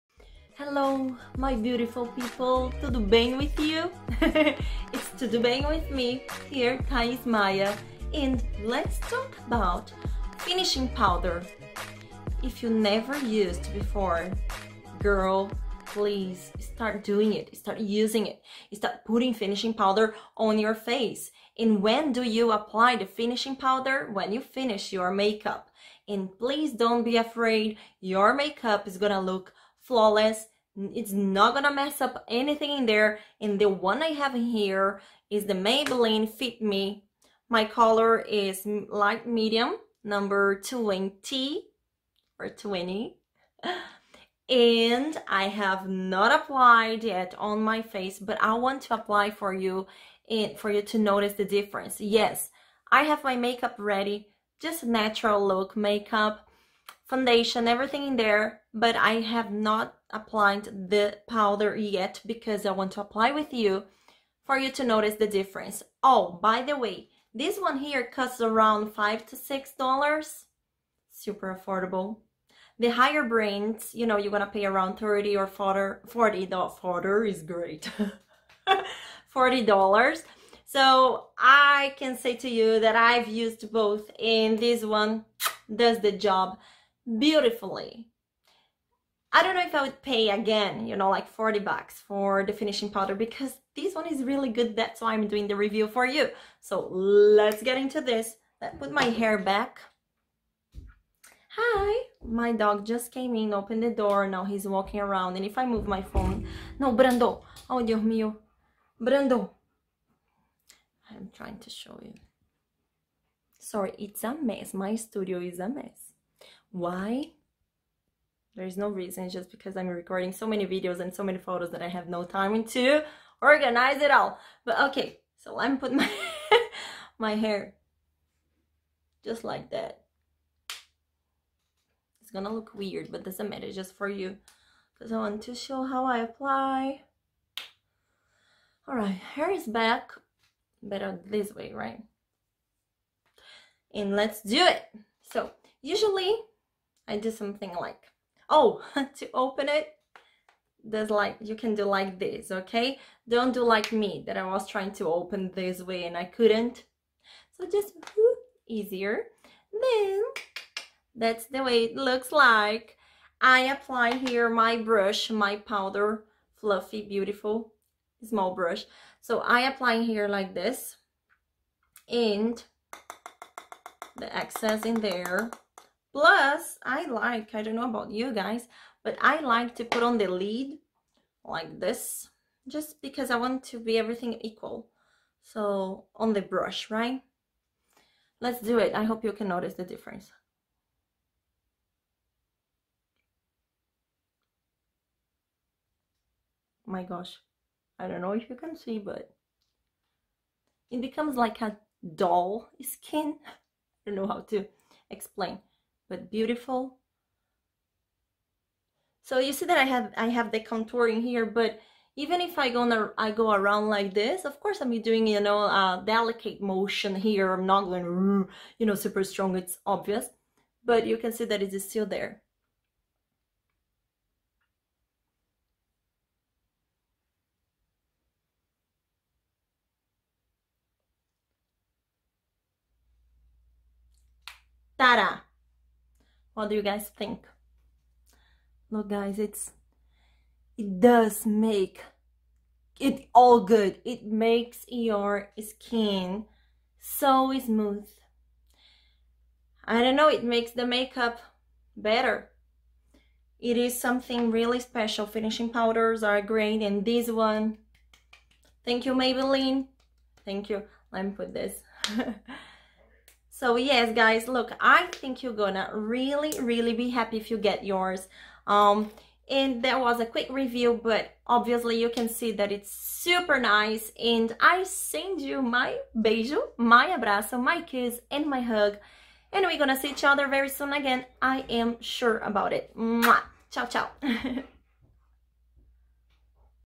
Best Drugstore Setting/Finishing Powder! - Combination Skin
I'm in love with this one, it's simply great - in price and effectiveness! My make up looks flawless and yes, it lasts for a longer time. You can get your Maybelline Finishing Powder at any drugstore or even on amazon ;)
Beijos e Abraços XoXo , ™

Your presence here matters so much to me and I already thank you for being here!

song by Squelinki